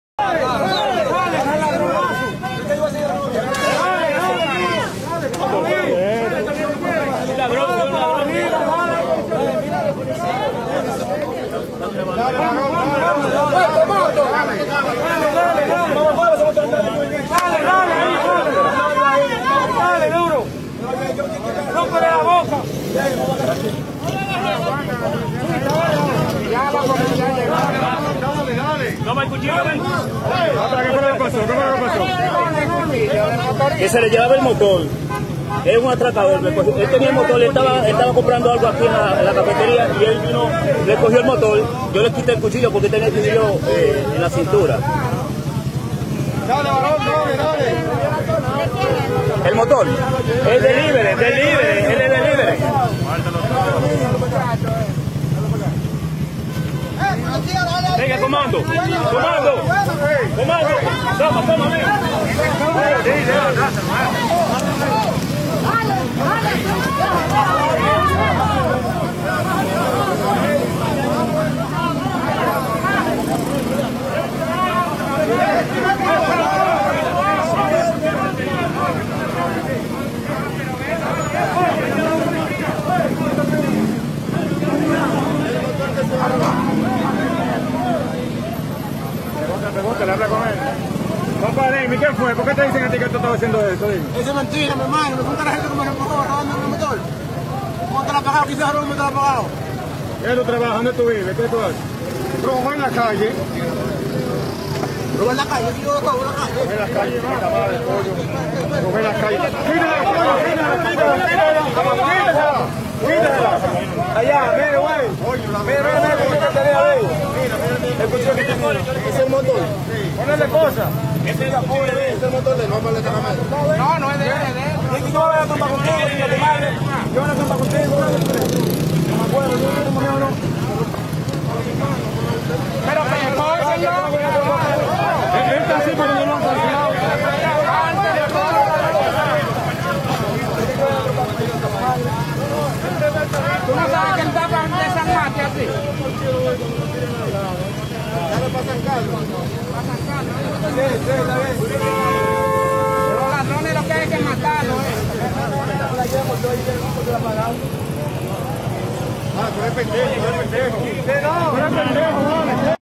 Zona Colonial Santo Domingo: Busted! A thief gets caught and slapped up!
Near Parque Independencia, Zona Colonial a large mob develops after a thief is captured and beaten after trying to evade arrest.

This is ViralDR your source of shocking content from the beautiful island of the Dominican Republic.